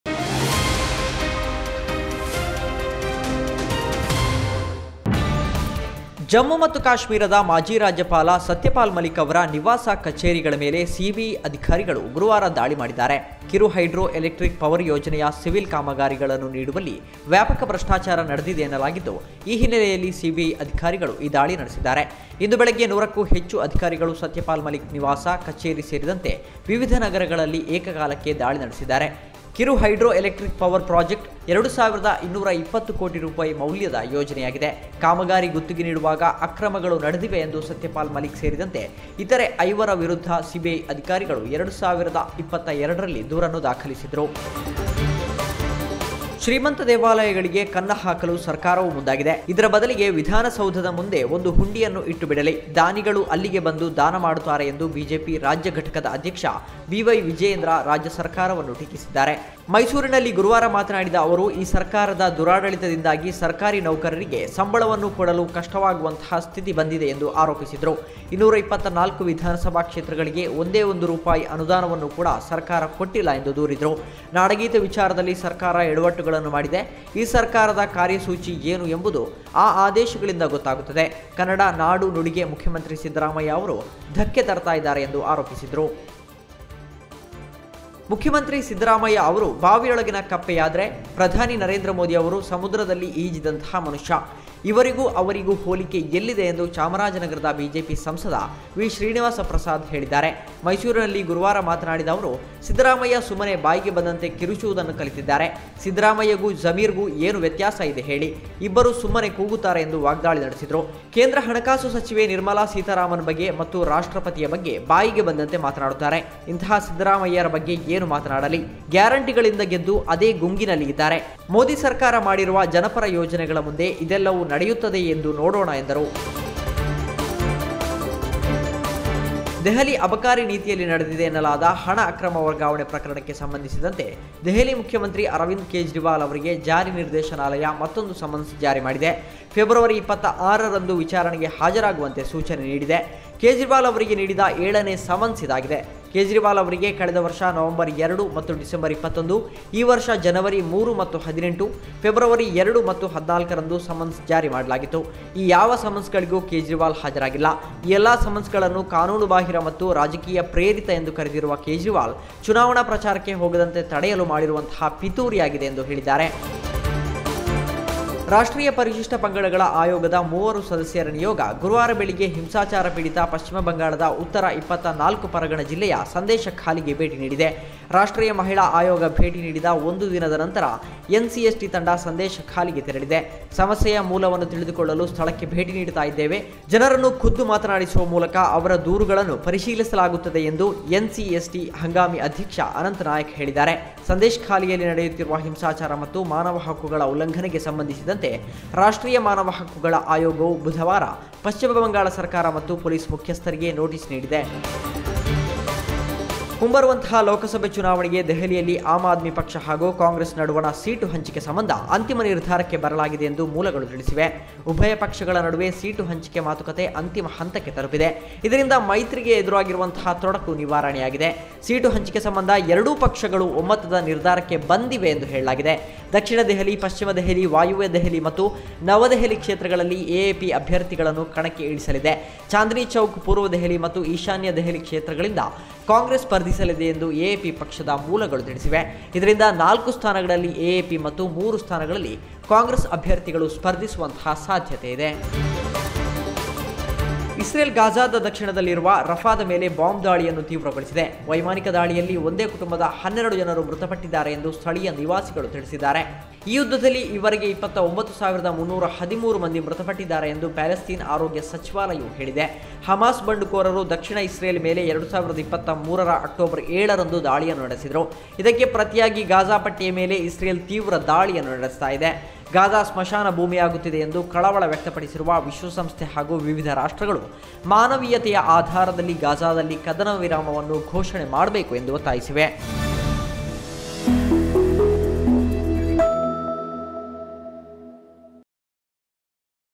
ಮಾಜಿ ರಾಜ್ಯಪಾಲ ಸತ್ಯಪಾಲ್ ಮಲಿಕ್ ನಿವಾಸದ ಮೇಲೆ ಸಿಬಿಐ ದಾಳಿ | Satyapal Malik | CBI
ಇಂದು ಬೆಳಗ್ಗೆ ನೂರಕ್ಕೂ ಹೆಚ್ಚು ಅಧಿಕಾರಿಗಳು ಸತ್ಯಪಾಲ್ ಮಲಿಕ್ ನಿವಾಸ, ಕಚೇರಿ ಸೇರಿದಂತೆ ವಿವಿಧ ನಗರಗಳಲ್ಲಿ ಏಕಕಾಲಕ್ಕೆ ದಾಳಿ ನಡೆಸಿದ್ದಾರೆ. ಕಿರು ಹೈಡ್ರೊ ಎಲೆಕ್ಟ್ರಿ ಕ್ ಪವರ್ ಪ್ರಾಜೆಕ್ಟ್ ₹2,220 ಕೋಟಿ ಮೌಲ್ಯದ ಯೋಜನೆಯಾಗಿದೆ. ಕಾಮಗಾರಿ ಗುತ್ತಿಗೆ ನೀಡುವಾಗ ಅಕ್ರಮಗಳು ನಡೆದಿವೆ ಎಂದು ಸತ್ಯಪಾಲ್ ಮಲಿಕ್ ಸೇರಿದಂತೆ ಇತರೆ ಐವರ ವಿರುದ್ಧ ಸಿಬಿಐ ಅಧಿಕಾರಿಗಳು 2022ರಲ್ಲಿ ದೂರು ದಾಖಲಿಸಿದ್ದರು.